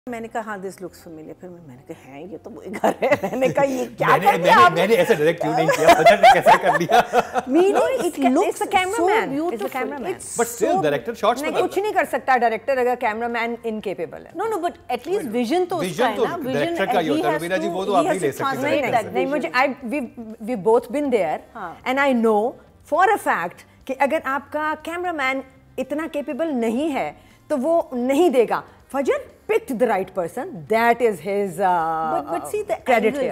How Is This Possible? l Burns Road Kay Romeo Juliet Drama Review l Kya Drama Hai
How Is This Possible? l Burns Road Kay Romeo Juliet Drama Review l Kya Drama Hai With Mukarram Kaleem

Burns Road Kay Romeo Juliet Synopsis:
Story about two individuals from different backgrounds who unexpectedly fall in love and fight for it. Iqra Aziz as Freeya is a free-spirited girl who is precious to her family. She belongs to a Memon family.

Channel: ARY Digital
Writer: Parisa Siddiqui
Director: Fajr Raza
Production: Big Bang Entertainment

Cast:
Iqra Aziz, Hamza Sohail, Shabbir Jan, Khalid Anum, Raza Samoo, Zainab Qayyum, Samhan Ghazi, Hira Umar, Shaheera Jalil Albasit

Judges: Rubina Ashraf, Waseem Abbas, Marina Khan